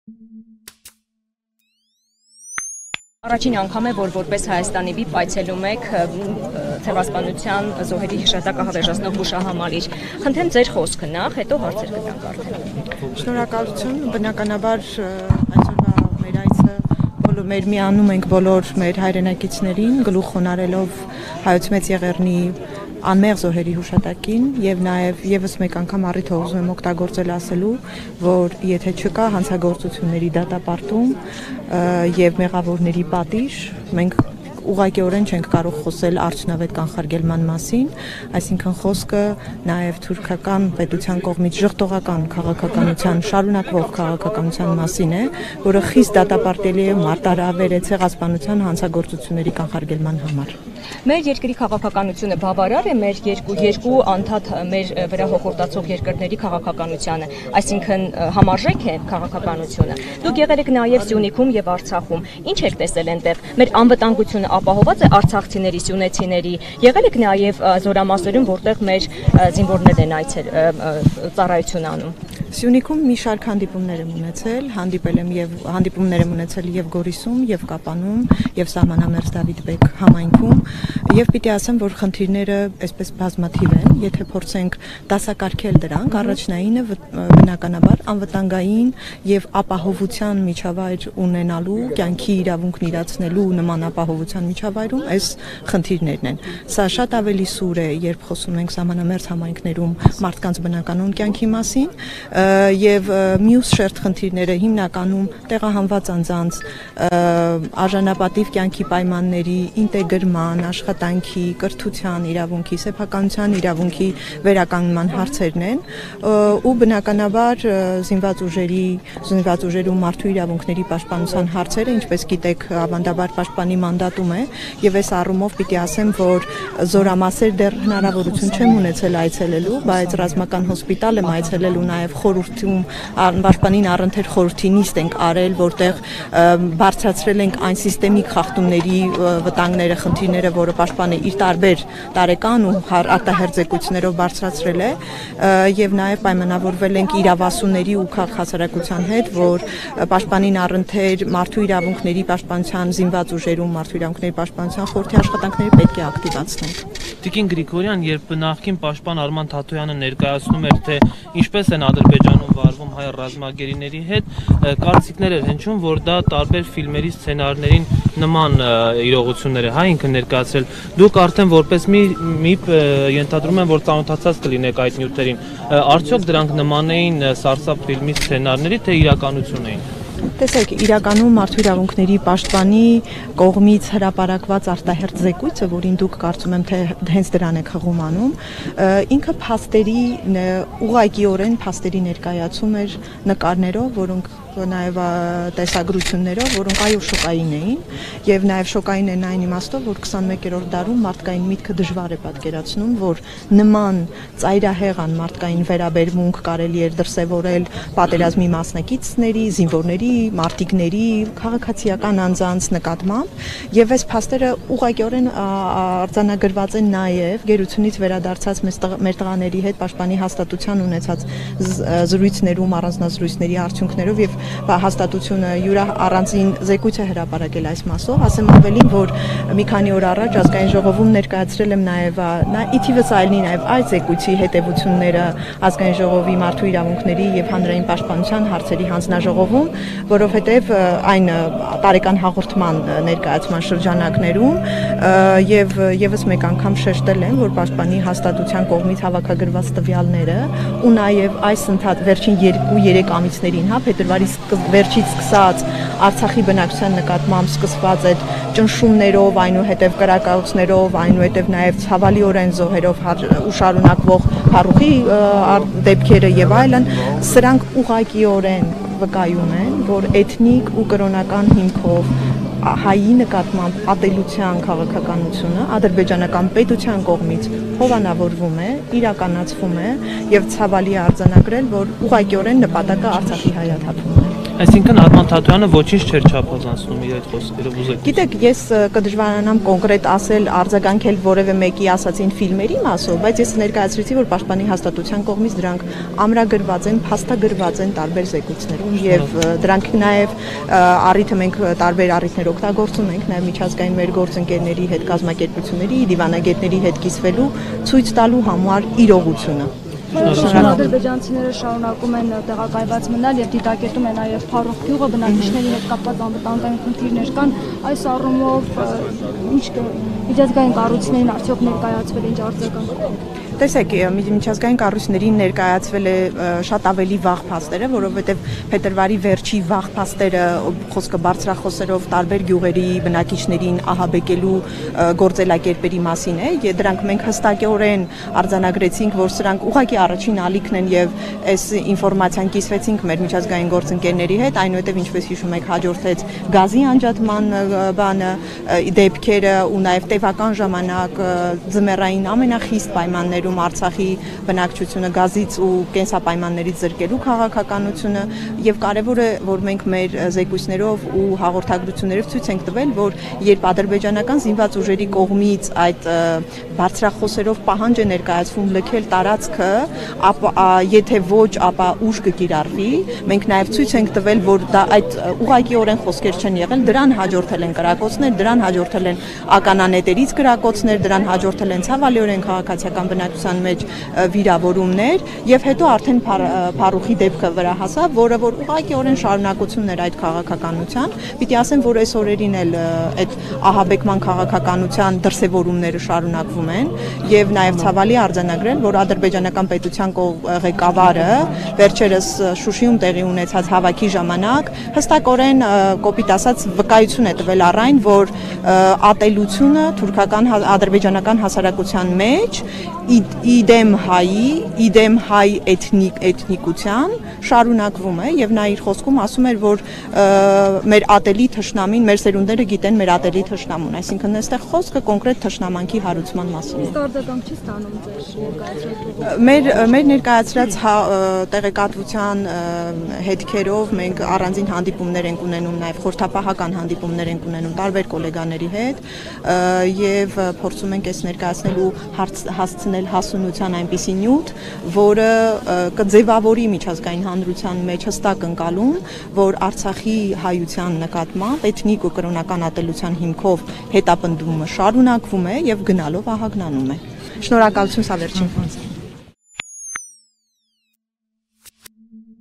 Արցախի հայության նկատմամբ էթնիկ ու կրոնական ատելության հիմքով հետապնդումը շարունակվում է. ՀՀ ՄԻՊ
Հայի նկատմամբ ատելության քաղաքականությունը ադրբեջանական պետության կողմից իրականացվում է, և նպատակն Արցախի հայաթափումն է. Քրիստինե Գրիգորյան